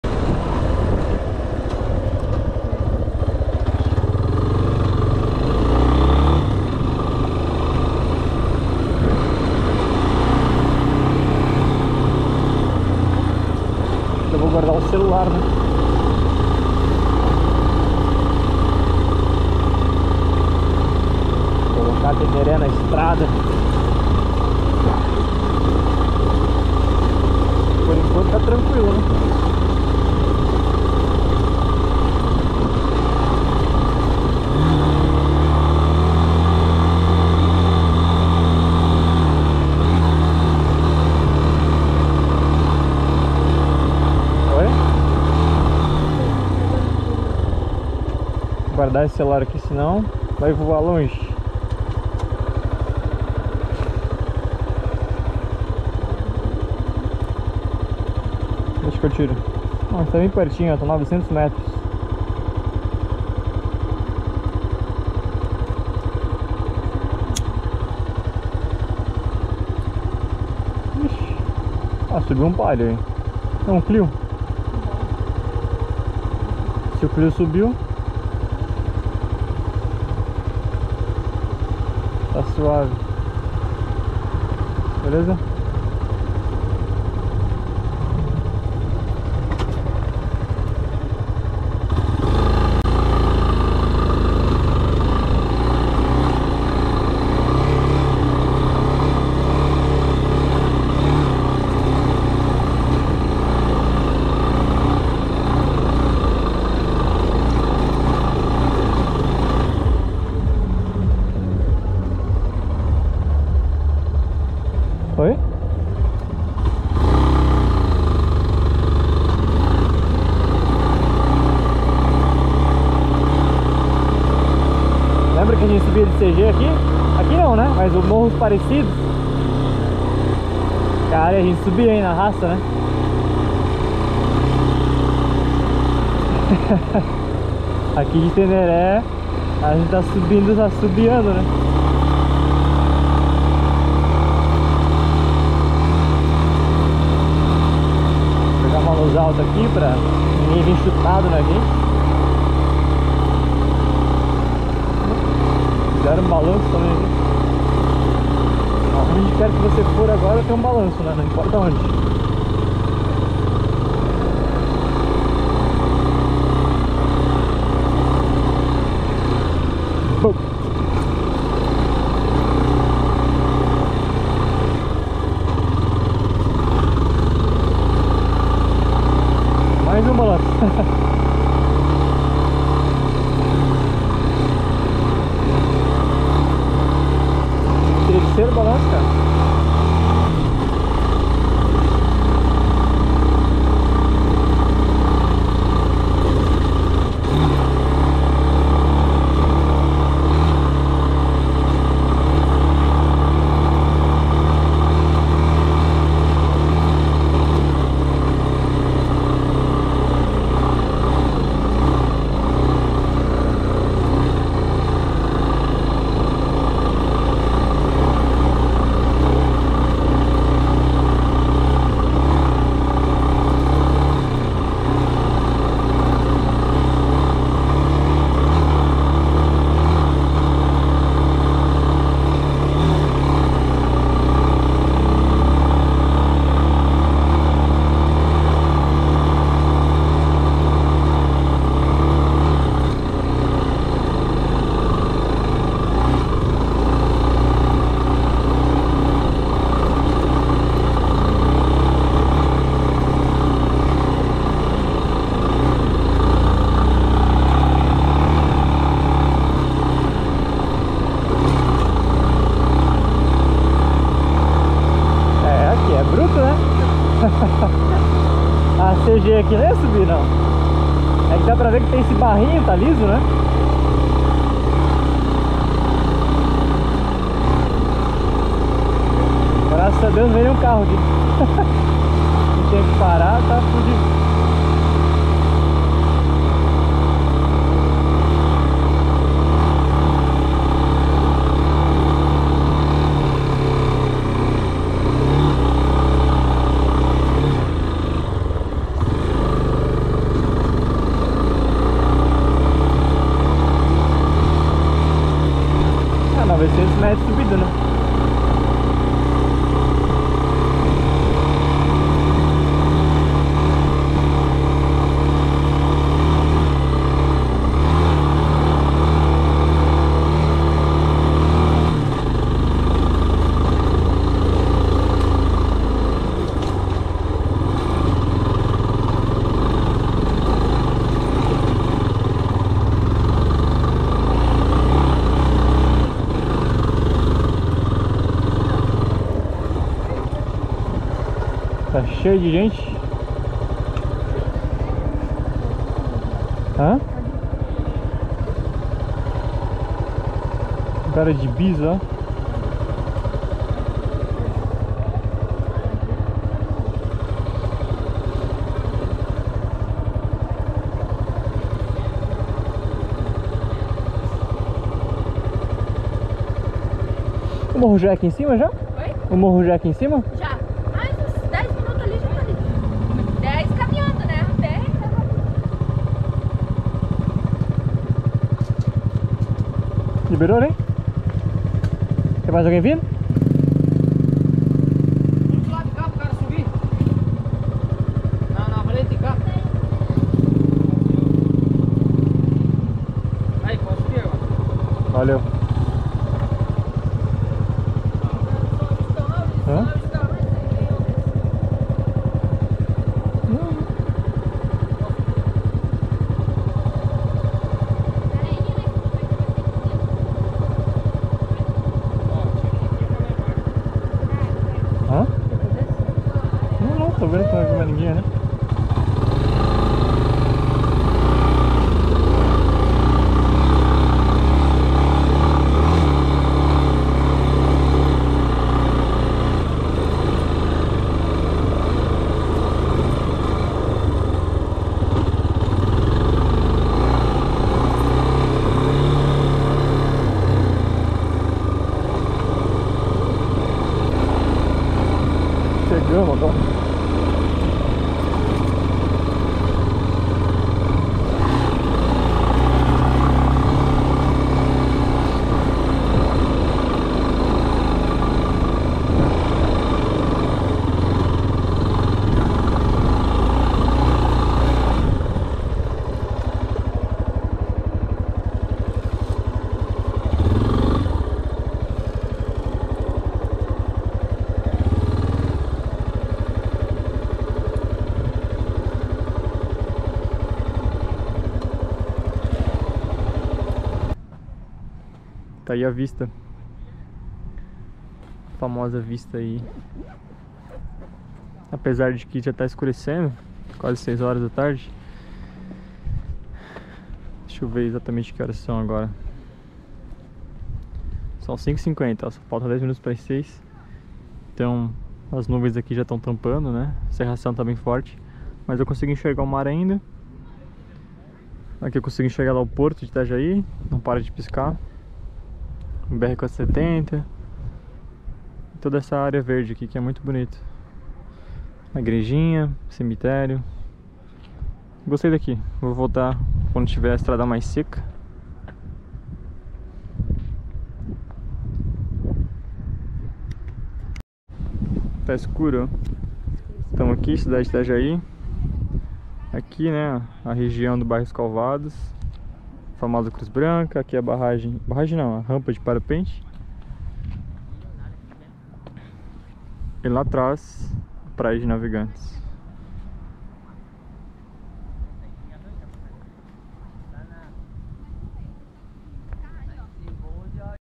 MORRO DA PEDRA TODO O TRAJETO ATÉ O TOPO DE MOTO
MORRO DA PEDRA TODO O TRAJETO ATÉ O TOPO
Siga @ocatarinense

[SIGA O CATARINENSE]